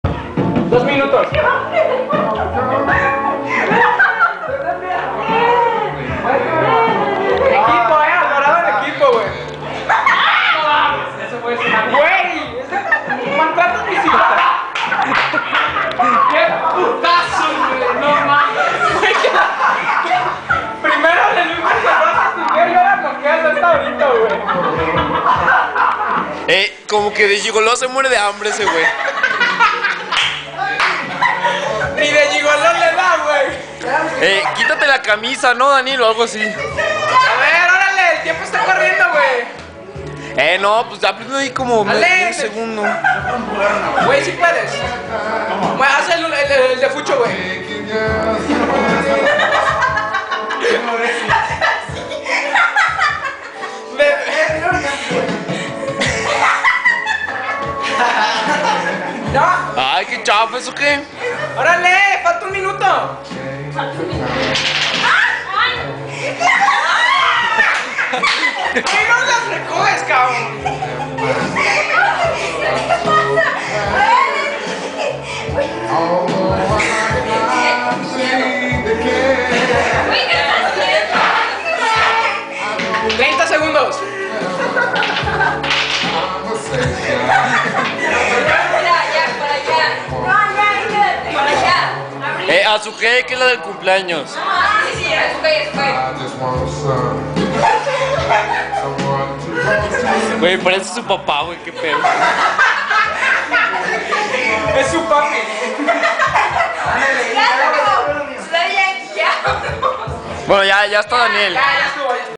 Gigodani feat. in "Hungry Gigolo 2: menage à trois"
Daniel volvio a perder en los juegos y ahora tiene q unir sus poderes seductivos con luis alias el teleton, a nuestro seductor le sale todo mal y termina volandole la cabeza a brito, definitivamente de gigolo...se MUERE DE HAMBRE!!